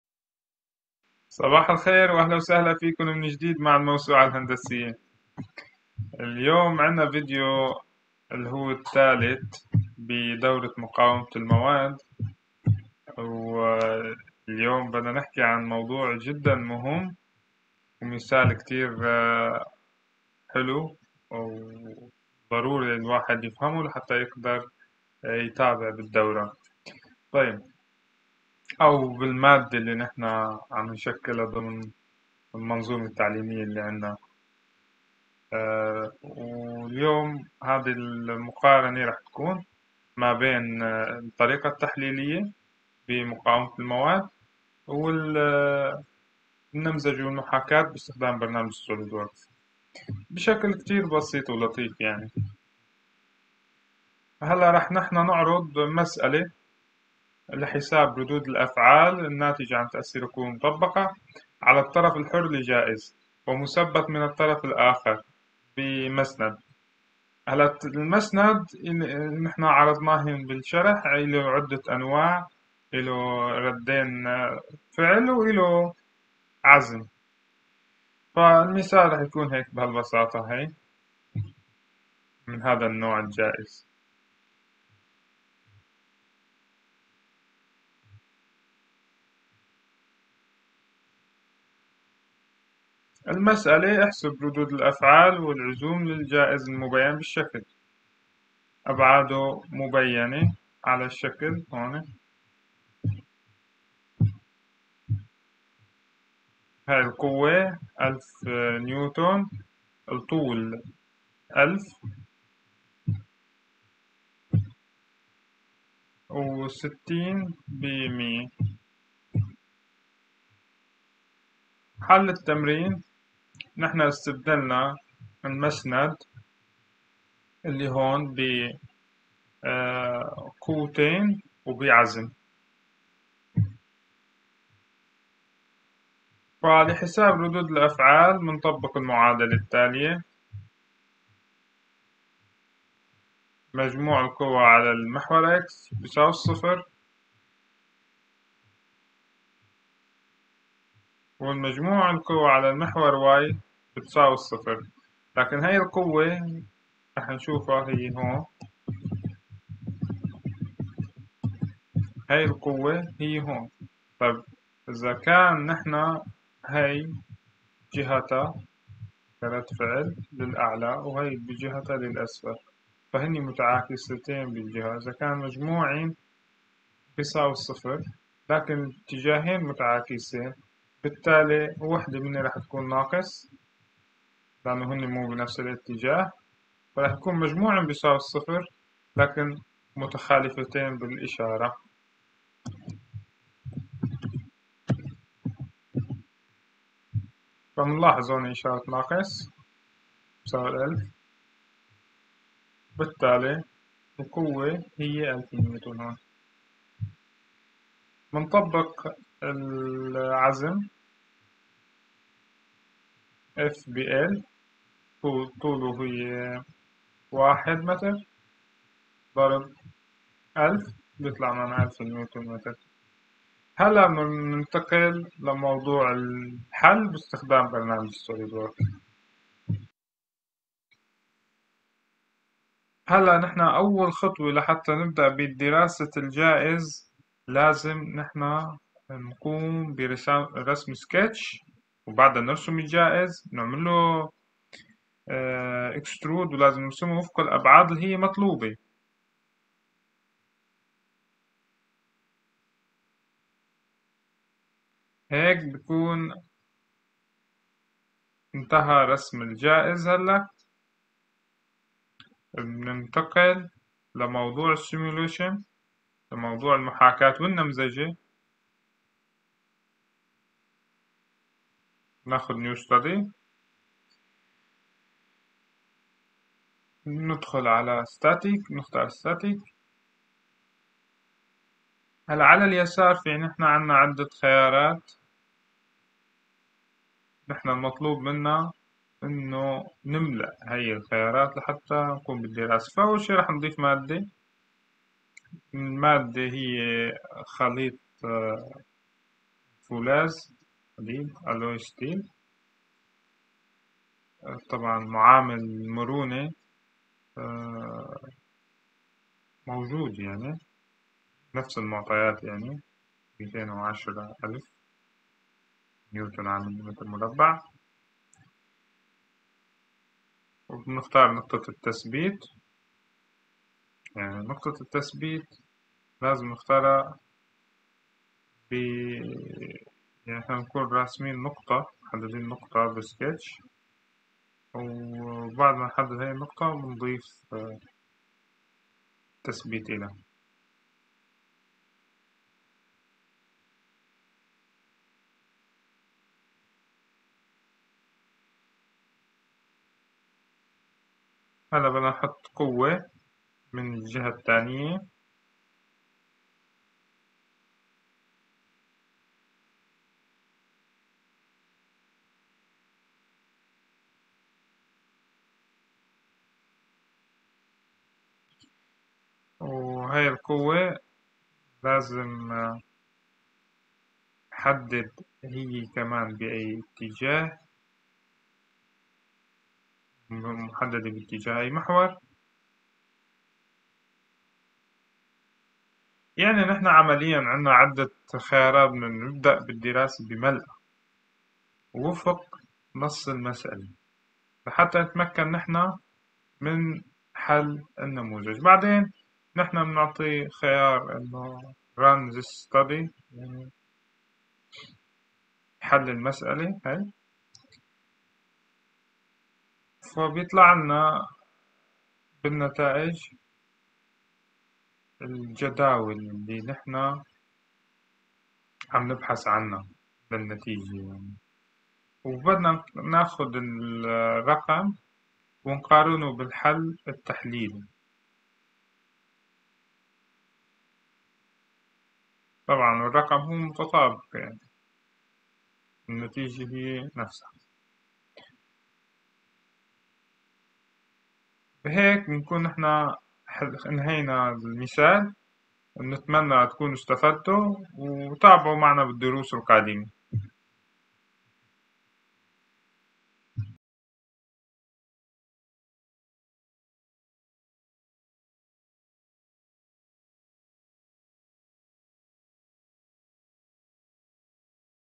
#003 The Third lecture on the resistance of materials
Third lecture
The Engineering Encyclopedia offers its role in the field of resistance to materials, modeling and simulation on Solidworks programs With the engineer  Mohammad wardeh
And how to deal with finite element analysis (FEA) which is a simulation of any specific physical phenomenon using a digital technique called the finite element method (FEM).
therefore This course displays exercises to calculate reactions, calculate stresses, draw the plans of forces and moments, and obtain results in an analytical manner with the presentation of the necessary laws and tables.
Then we solve the exercise on the Solidworks program
(Simulation in SolidWorks—static study)
and We compared the results between the analytical method and the modeling method .
In the fourth lecture, we will present to you the third exercise, which is beam, fixed on both sides and exposed to a force in the middle. Then we calculate the reactions and the resulting moments.

● (Adobe Photoshop 2021) in the thumbnail
●  (  Blender - ZD Soft Screen Recorder - sony vegas )
ــــــــــــــــــــــــــــــــــــــــــــــــــــــــــــــ
ــــــــــــــــــــــــــــــــــــــــــــــــــــــــــــــ
ــــــــــــــــــــــــــــــــــــــــــــــــــــــــــــــ
Telgram:
ــــــــــــــــــــــــــــــــــــــــــــــــــــــــــــــ
ــــــــــــــــــــــــــــــــــــــــــــــــــــــــــــــ
ــــــــــــــــــــــــــــــــــــــــــــــــــــــــــــــ
ــــــــــــــــــــــــــــــــــــــــــــــــــــــــــــــ
ـــــــــــــــــــــــــــــــــــــــــــــــــــــــــــــ
ــــــــــــــــــــــــــــــــــــــــــــــــــــــــــــــ
ـــــــــــــــــــــــــــــــــــــــــــــــــــــــــ
ــــــــــــــــــــــــــــــــــــــــــــــــــــــــــ
Hashtags Engineering Encyclopedia
#ِEng_Abd_Alzem_Hamadeh